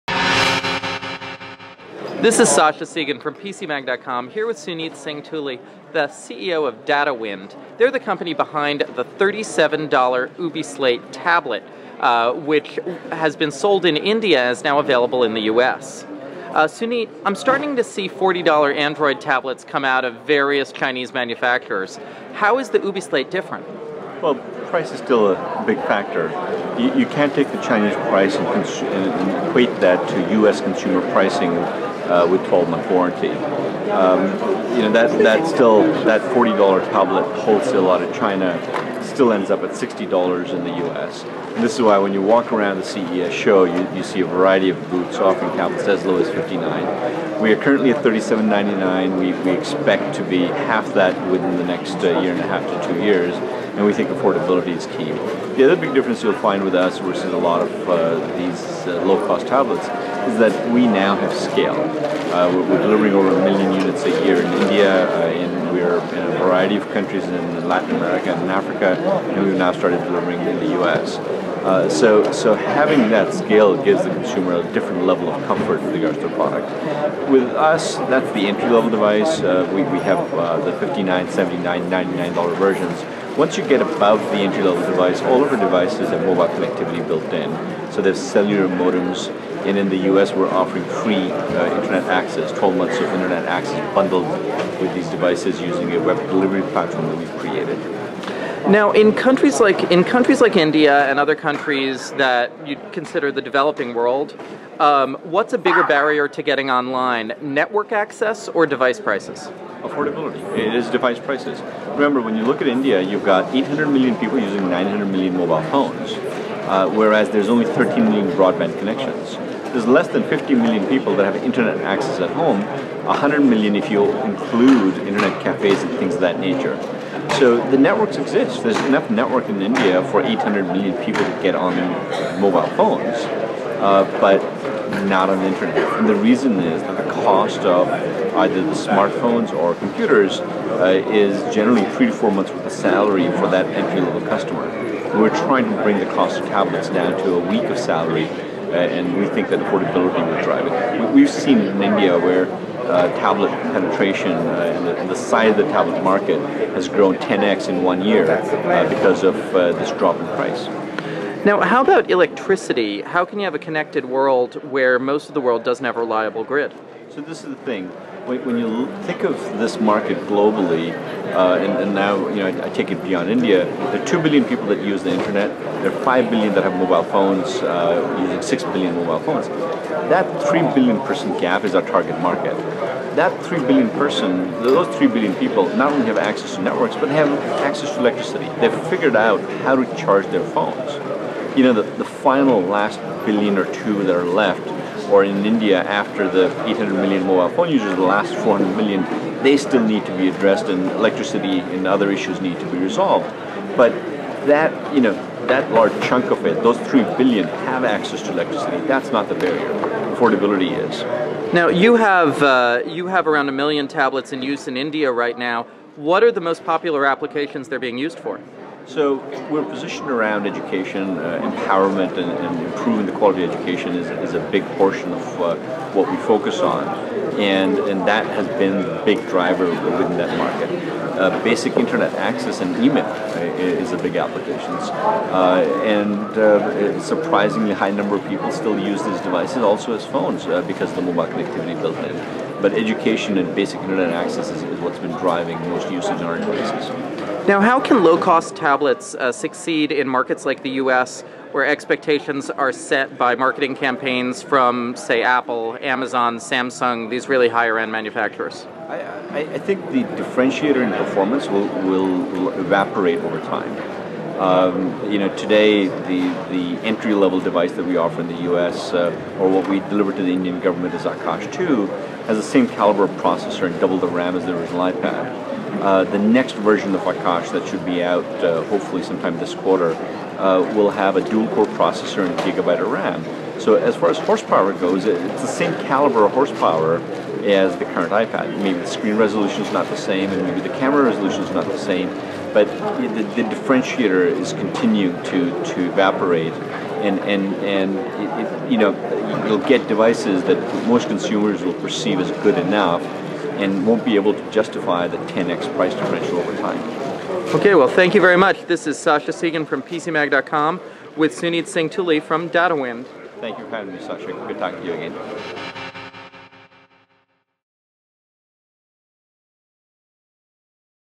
5 Questions with Suneet Singh Tuli, CEO of Datawind - CES 2014
Datawind makes one of the world's cheapest Android tablets, and they've sold nearly a million of them in India. We ask them how they'll get the next billion people online.